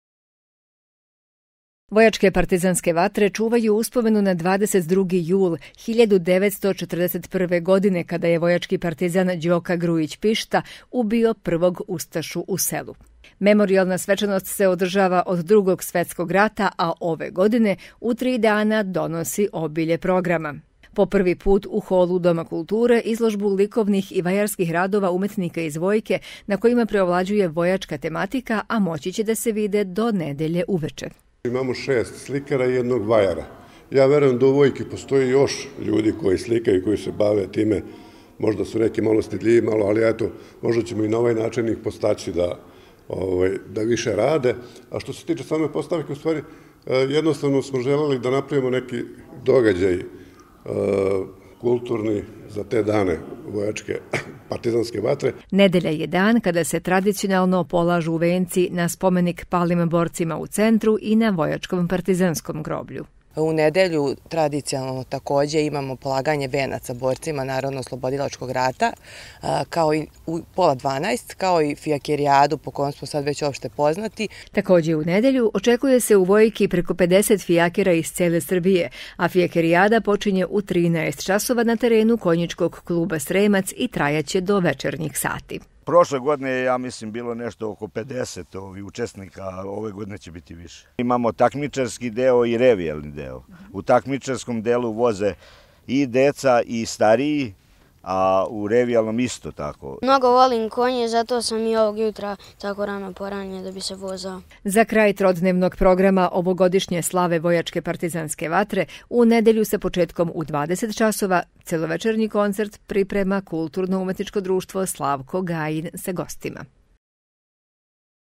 Za vikend Vojačke partizanske vatre
Od  večeras do nedelje u holu Doma kulture u Vojki  će otvorena izložba radova vojačkih likovnih stvaralaca, povodom vojačke partizanske slave, koja donosi još nekoliko kulturno – zabavnih programa.   U nedelju će na centru na spomenik palim borcima i na vojačkom partizanskom groblju biti položeni venci, istog dana  od 13 časova na terenu konjičkog kluba Sremac uslediće fijakerijada a u večernjim satima u nedelju koncert KUD-a  Slavko Gajin sa gostima.